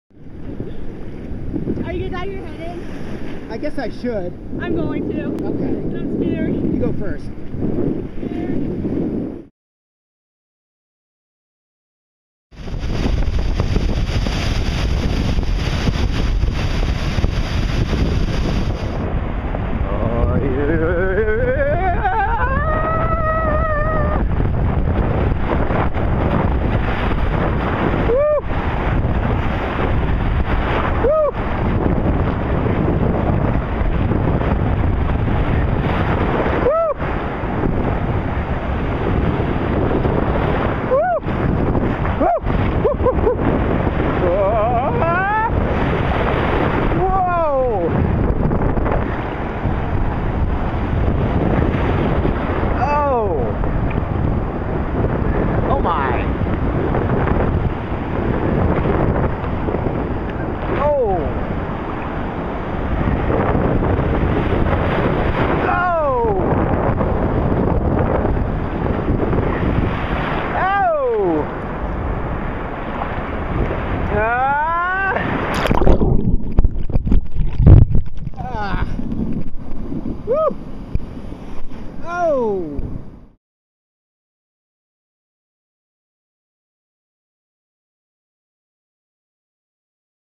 90 Second Escape: First Dip of the Season
Spring break and who can ignore the call of the beach — even when the water temperature is still a wintry 64?